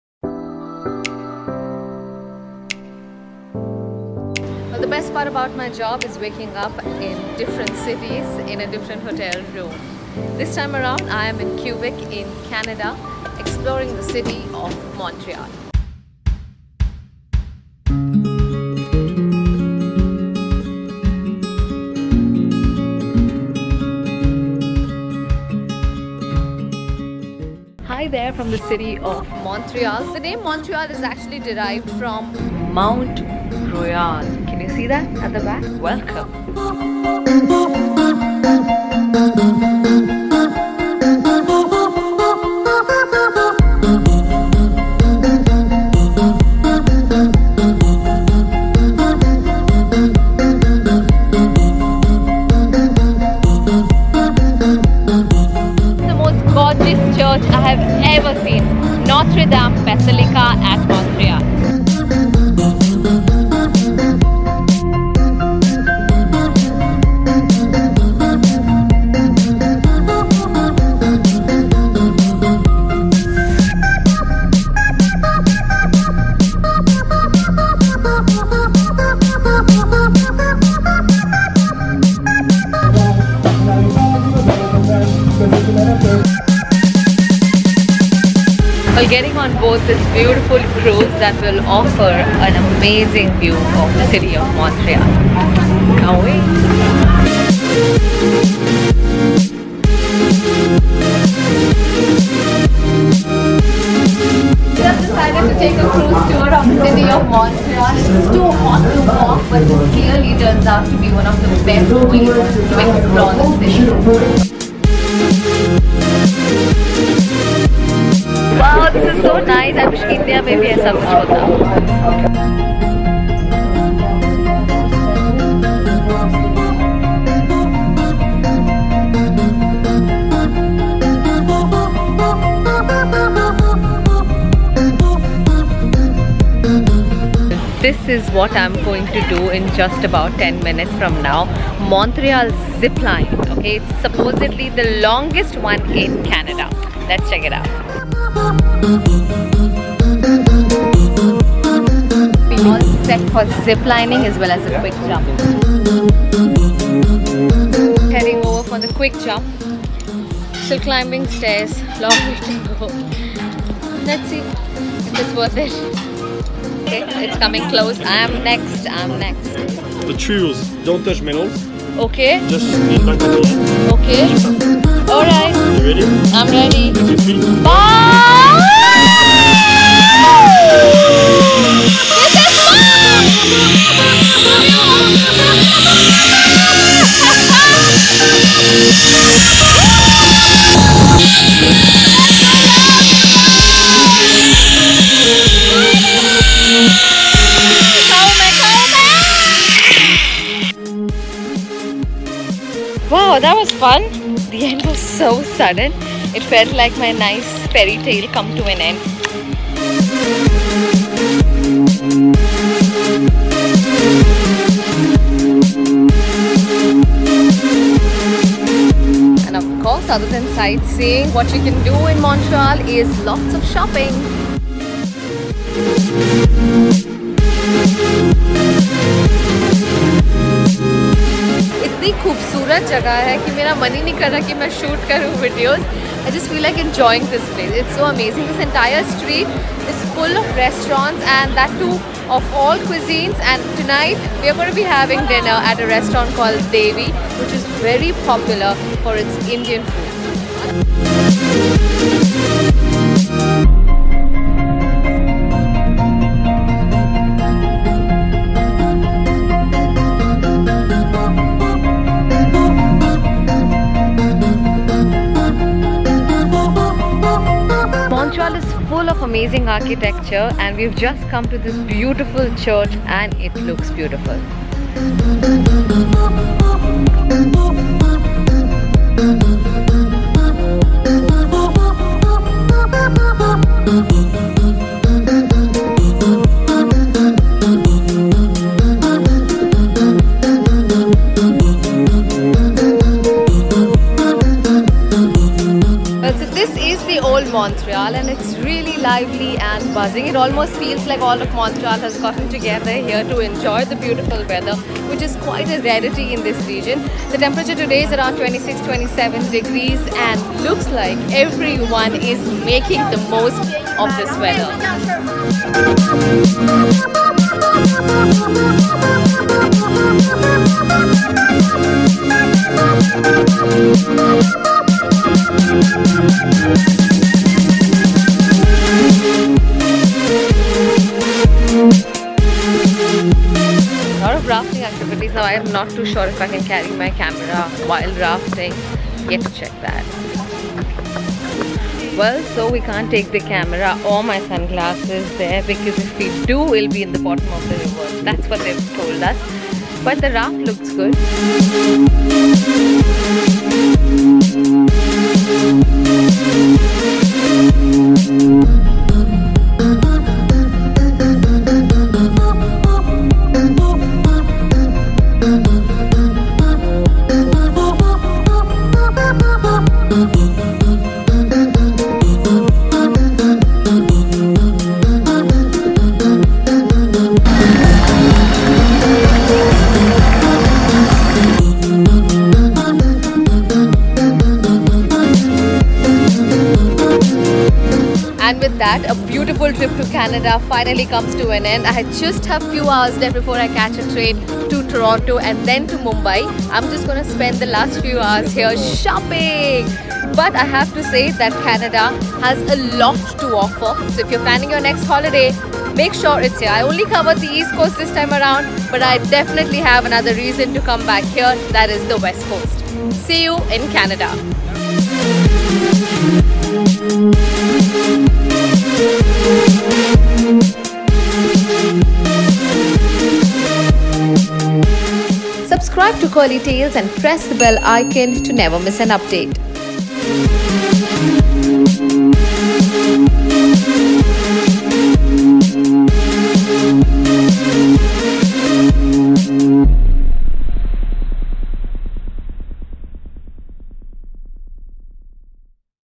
Always Trippin' Episode 5 - Top 9 Things To Do In Montreal, Canada | Curly Tales
Montreal is where America meets Europe. Here's your itinerary for 48 hours! Get atop of an observation deck or on a guided cruise tour. Curly Tales makes a checklist for you for your next visit to Montreal.